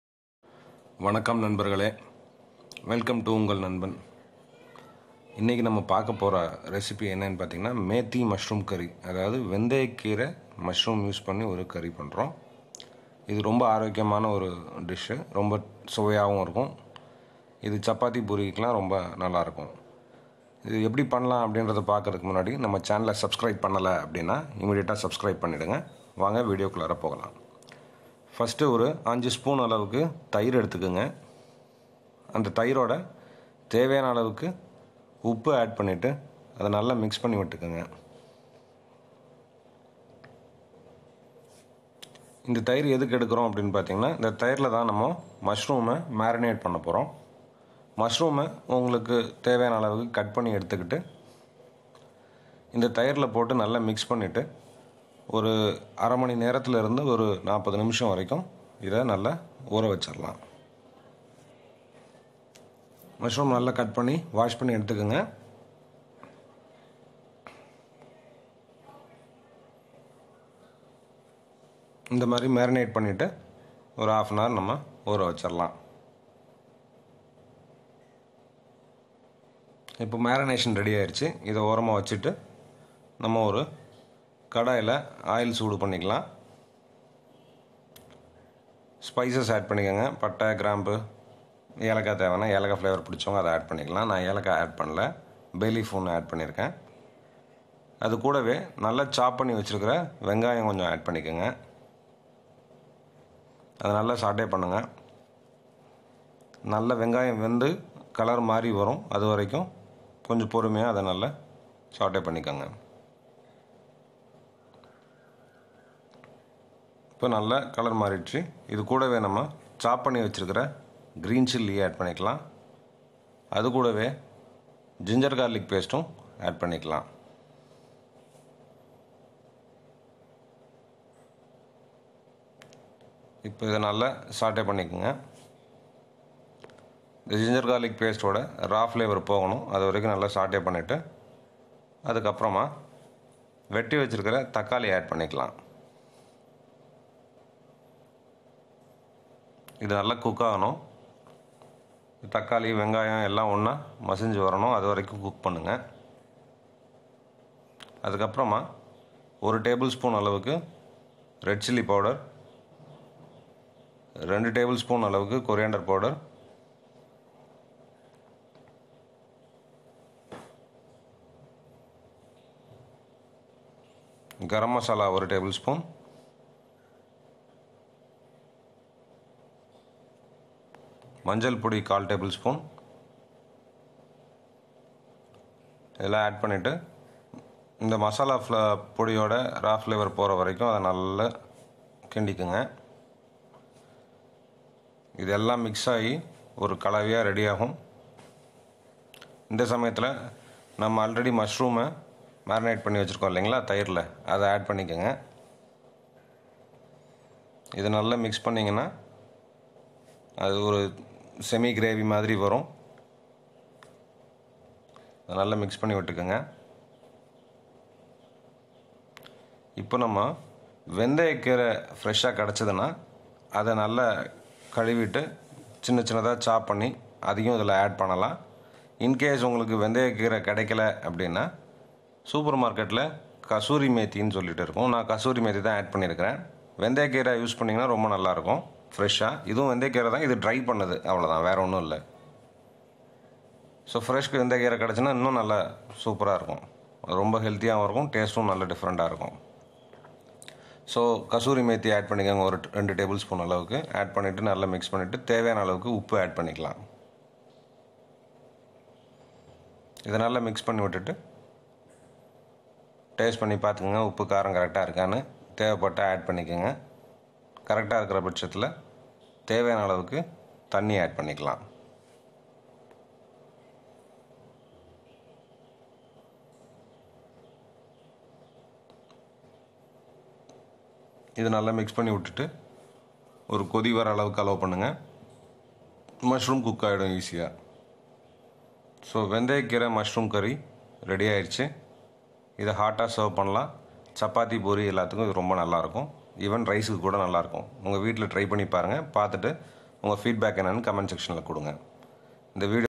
ஆரோக்கியம் தரும் மேத்தி (வெந்தய கிரை) காளான் கறி ஒரு முறை சுவைத்து பாருங்கள் / Methi Mushroom Curry
Dear Viewers,
Welcome to Ungal Nanban - Sharing is Caring

Today's Recipe: Methi Mushroom Curry

Watch the Video till the end to know step by step Process

Ungal Nanban